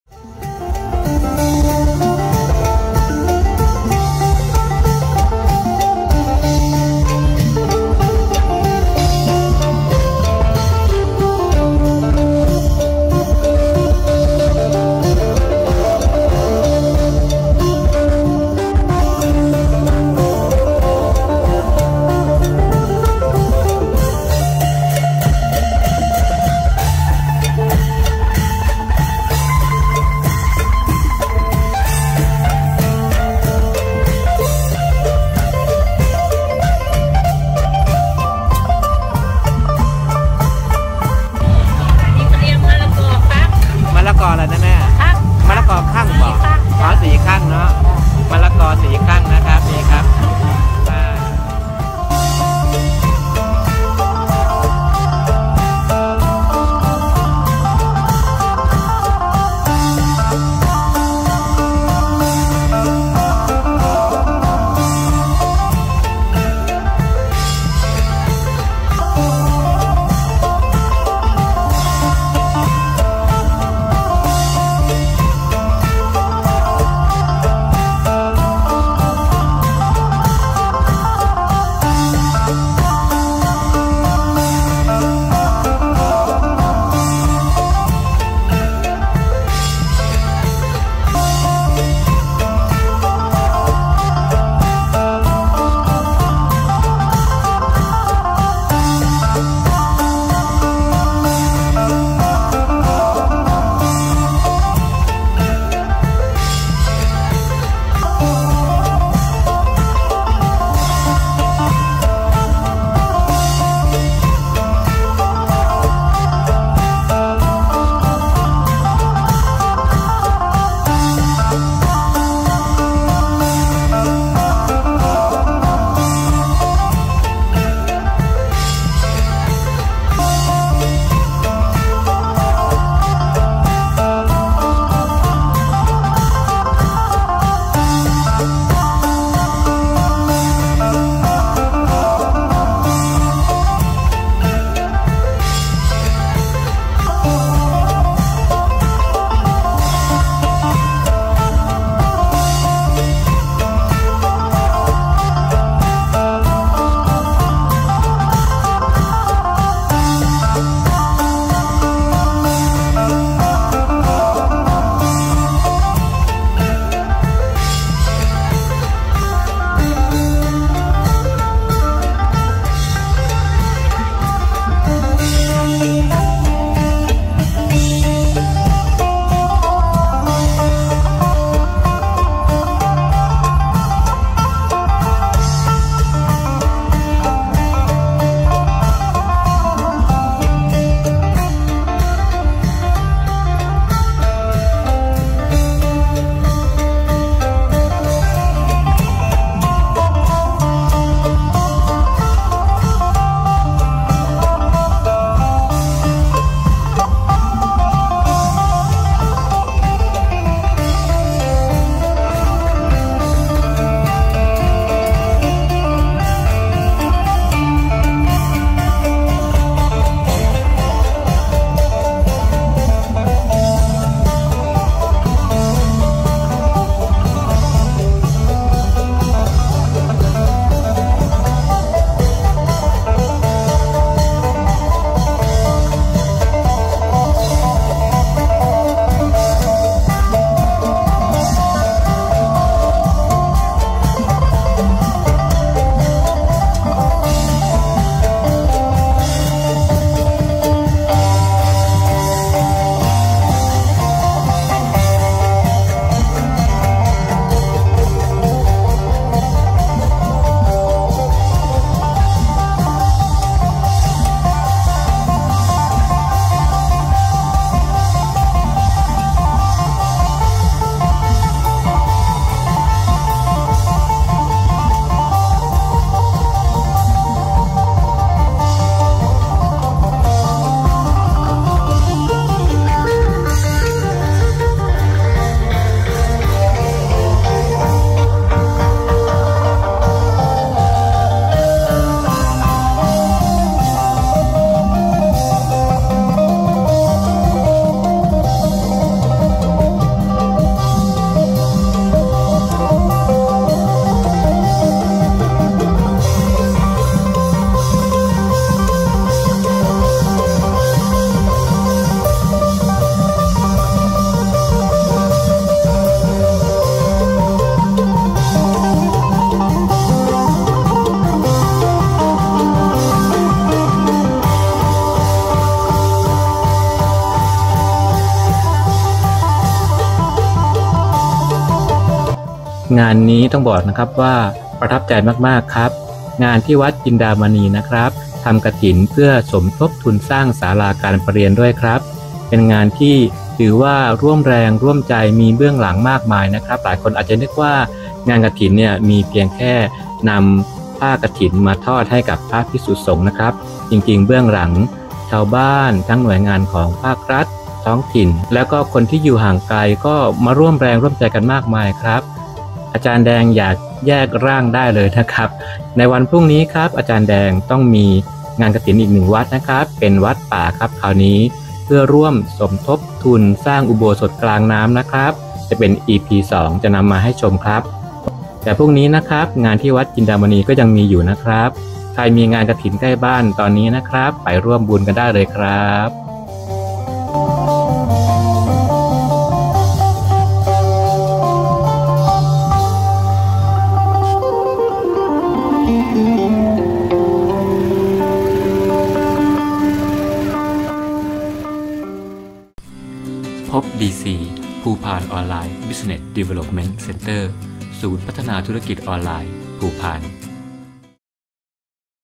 ทอดกฐินสามัคคี ขบวนแห่กฐิน วัดจินดามณี จ.สกลนคร ใน EP1 ,ถวายผ้ากฐิน วัดป่าหนองทัน ใน EP2 โดยพบดีซี
ทอดกฐินสามัคคี ขบวนแห่กฐิน กับสิ่งที่มากกว่าที่คิด ณ วัดจินดามณี อ.พรรณานิคม จ.สกลนคร วันที่ 26 ตุลาคม 2567 คือ EP1 ส่วนวันพรุ่งนี้ อ.แดง มีบุญกฐิน งานชนกันครับ จึงขอทำ EP2 เน้นงานพิธีสงฆ์ การถวายผ้ากฐิน ที่วันป่าหนองทัน จะรวมบรรยากาศให้เห็นงานประเพณีที่สำคัญ ซึ่งเกิดขึ้นทั่วไทย และ ในภาคอีสาน หรือ ภาคตะวันออกเฉียงเหนือ มีความศรัทธา ต่อพระพุทธศาสนาเป็นอย่างยิ่ง มีโรงทาน ธงกฐิน อีกทั้ง ชาวบ้าน ทั้งภาครัฐ และ ชุมชน ใกล้ไกล ร่วมมือร่วมใจกัน อุ่นหนา อิ่มบุญ น่าชื่นใจเป็นอย่างยิ่งครับ ผมถือว่าโชคดีที่ได้อยู่ในบรรยากาศ แห่กระฐินเข้าวัด ได้เห็น ได้เก็บภาพถ่าย และวีดีโอที่งดงามมาฝากทุก ๆ ท่าน การทอดกฐิน ไม่ว่าจะเป็น กฐินหลวง หรือ กฐินสามัคคี จะจัดขึ้นภายใน 1 เดือน ช่วง แรม 1 ค่ำเดือน 11 ไปจนถึงวันขึ้น 15 ค่ำเดือน 12 เพื่อ ถวายผ้ากฐิน ให้แก่พระภิกษุ พร้อมกับของบริวาร  วีดีโอ โดย อ.แดง พบดีซี PobDC